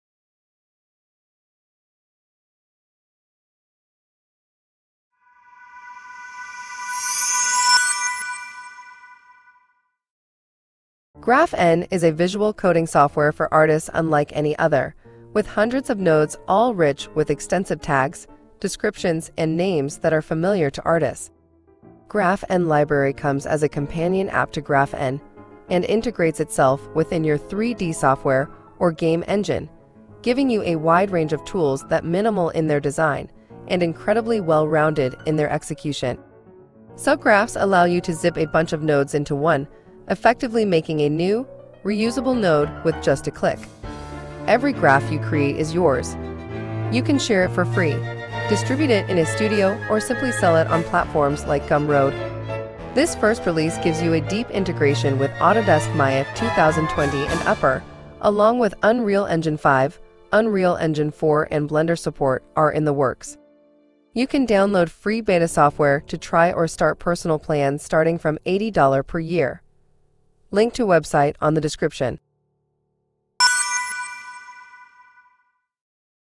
GraphN for Maya Unreal Engine and Blender | A visual coding software for Artists
Introducing GraphN for autodeskMaya UnrealEngine and Blender, a visual coding software for Artists